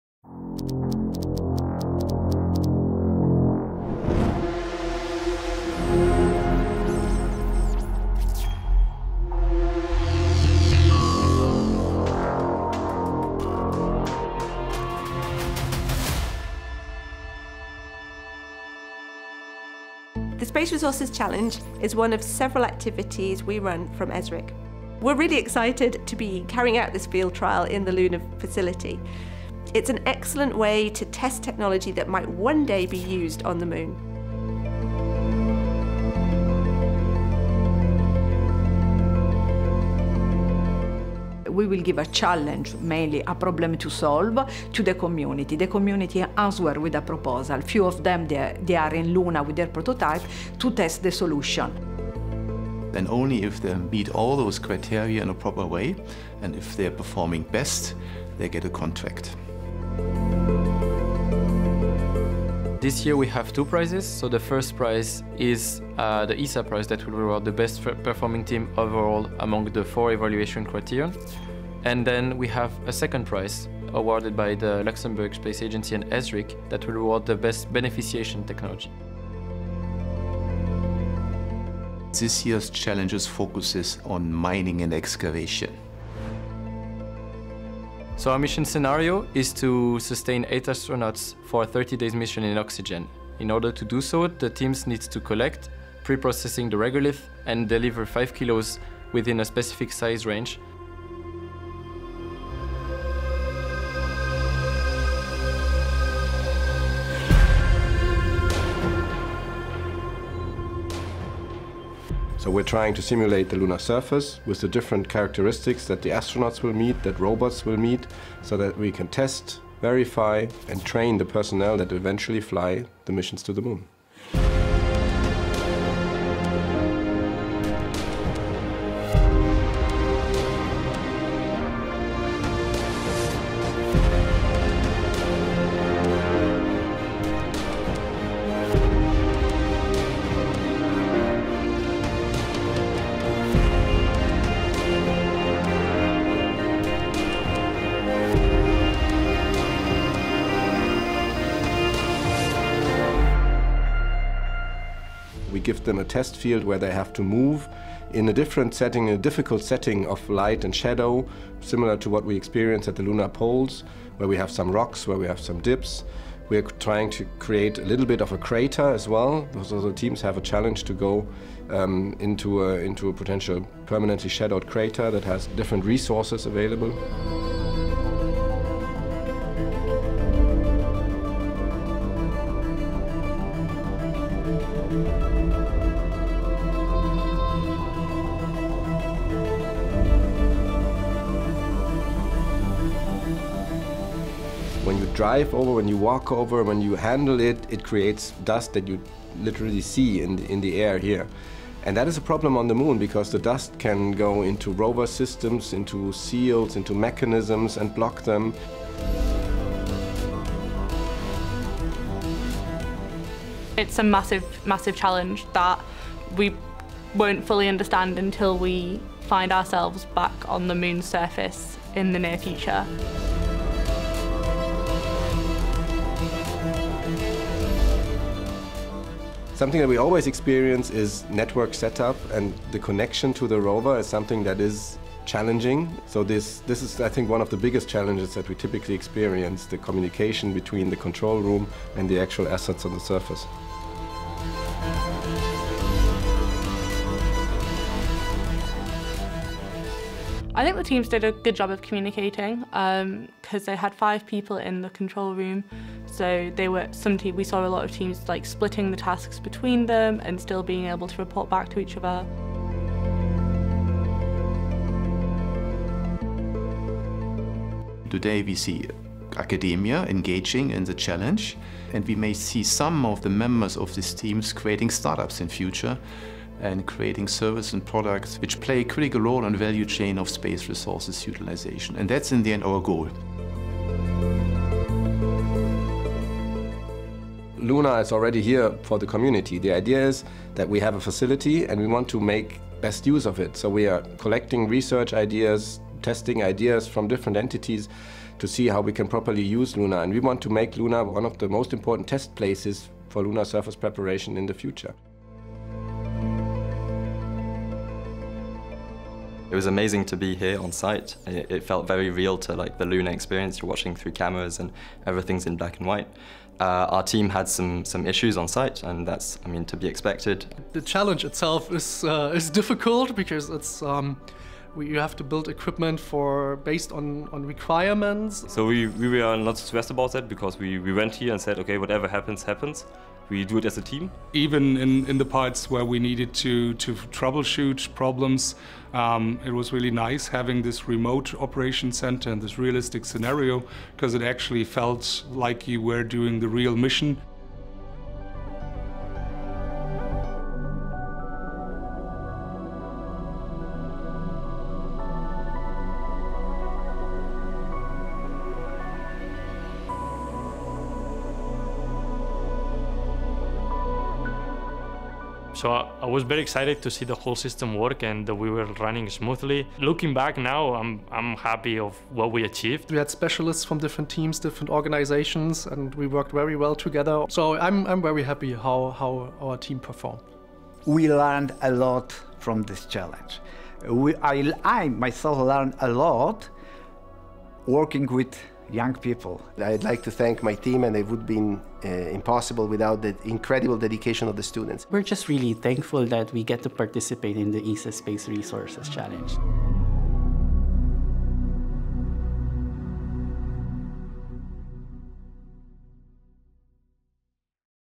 ESA-ESRIC 2nd Space Resources Challenge Field Trial
Field Test at LUNA (EAC, Cologne) from October 13-17, 2025
After a rigorous evaluation process, our evaluation board from ESA, ESRIC, and LSA has selected the eight teams that advanced to the second phase of the 2nd Space Resources Challenge, focusing on Excavation and Beneficiation technologies for Space Resources.

Congratulations to the following teams:

Team BREMEN – DLR & DFKI
Team Imperial Planetary Robotics Lab – Alumni Project Initiative – IPRL
Team LuMA – Maana Electric, SpaceR & Aalborg University
Team FZI DUST – Forschungszentrum Informatik
Team AGH Lunar Resources Initiative – AGH University of Krakow, SpaceTeam AGH, AGH Space Systems, CleverHive Space, PIAP Space & CBK PAN
Team CRADLE – Amentum & The University of Manchester
Team ASTROLITH –  Polytechnique Montreal
Team TUBular – TU Berlin